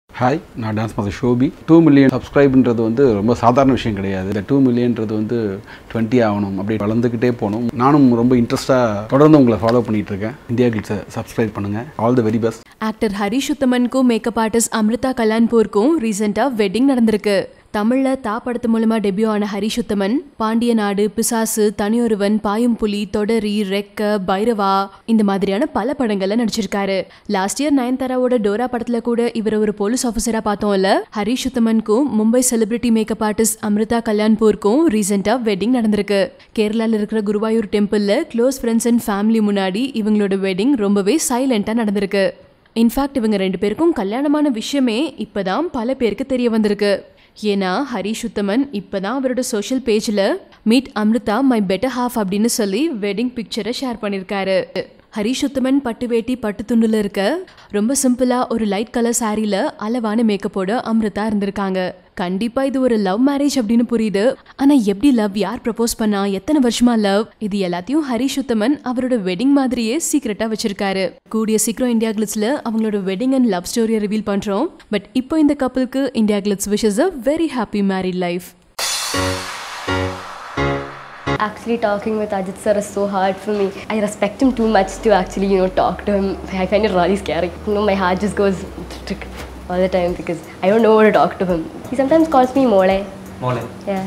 Actor Harish Uthaman Ties the Knot with Amrita Kalyan | Hot Cinema News
Harish Uthaman, who played pivotal roles in Bhairavaa, Thodari, Dora etc. tied the knot with his girlfriend Amrita Kalyan.